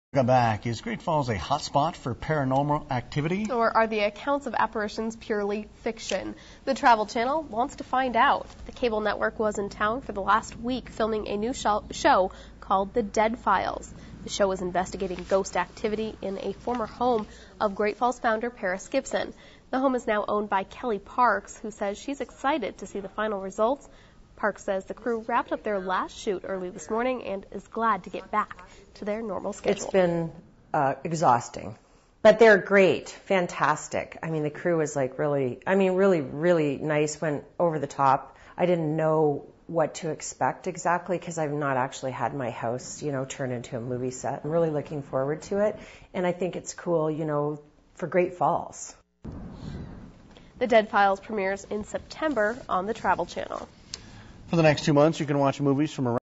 "The Dead Files" in Great Falls
The Travel Channel sent a team to Great Falls in April 2011 to film the segment for the show; the duo investigated alleged ghost activity in the former home of Great Falls founder Paris Gibson.